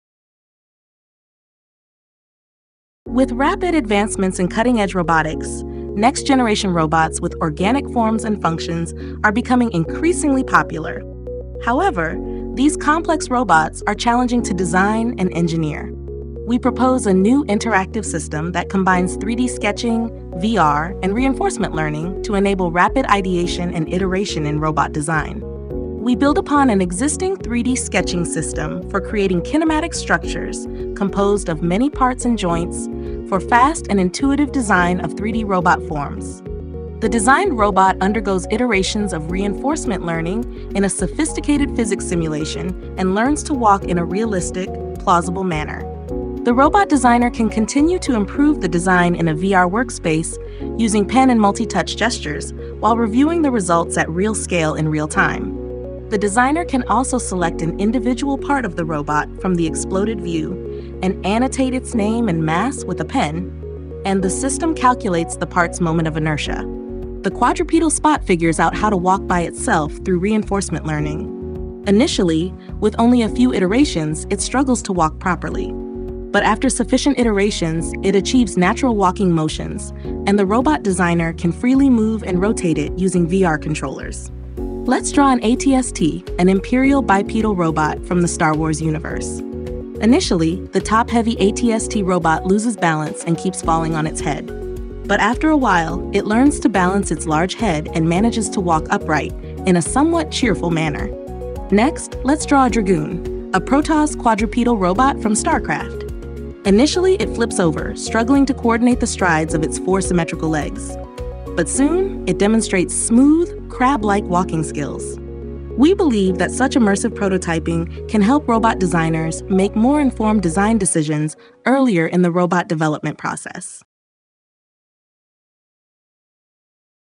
RobotSketch (SIGGRAPH 2024 Emerging Technologies) - 2 min. walk-through
SIGGRAPH '24 Emerging Technologies
RobotSketch: an interactive showcase of superfast design of legged robots

Robots consisting of many articulated parts performing complex movements are challenging to design. We showcase an interactive system for exploring shapes and structures of robots through 3D sketching, generating plausible movements of robots through AI, and reviewing and refining them in VR. Such immersive prototyping in the early stages can help reduce the time and cost associated with trial and error in later stages, contributing to shortening and streamlining of the robot development process.